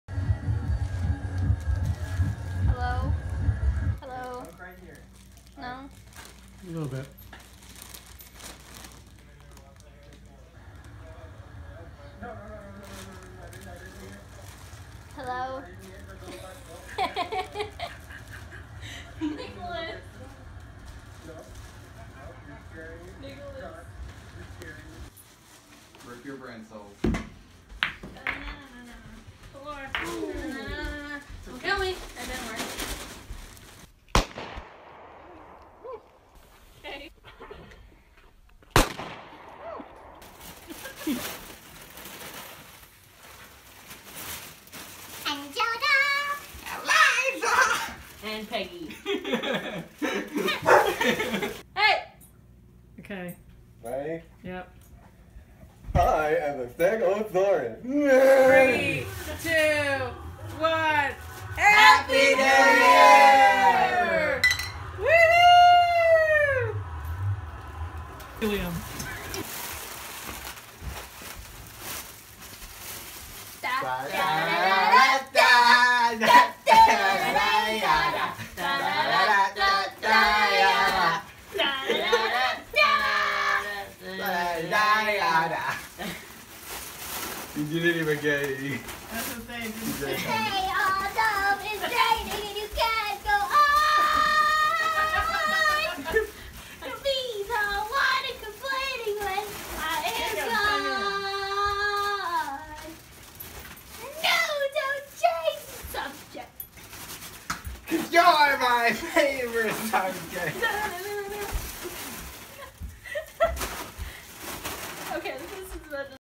It's Not New Years - New Years Helium Footage // RiskInsanity
Due to my tight schedule and Jake not being around I am posting this filler video! Enjoy a bunch of craziness from The Staley Manor!! Next weeks video will also, most likely, be a filler.